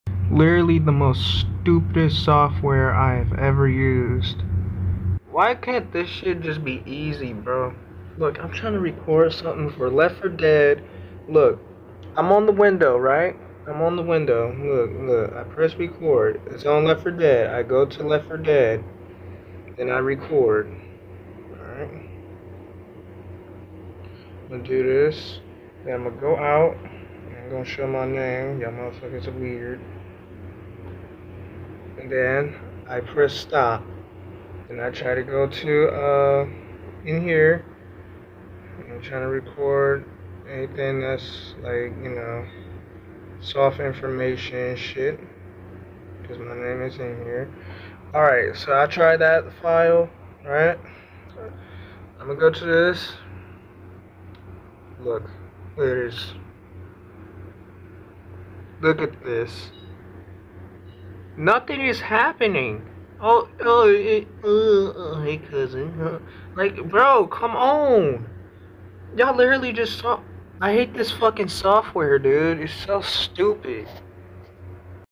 OBS sucks
hate this damn software so much it just randomly doesn't wanna do what the fuck it  Supposed to do, I click the window that i want to record and it just gives me a black screen for no fucking reason. then it wanna fucking lag even tho i dont even have it on high settings. I just wanna record gameplay of games that I wanna play but can't because of this stupid fucking software.

and no I am not gonna waste my money on something As simple as a fucking screen recorder.

fuck you OBS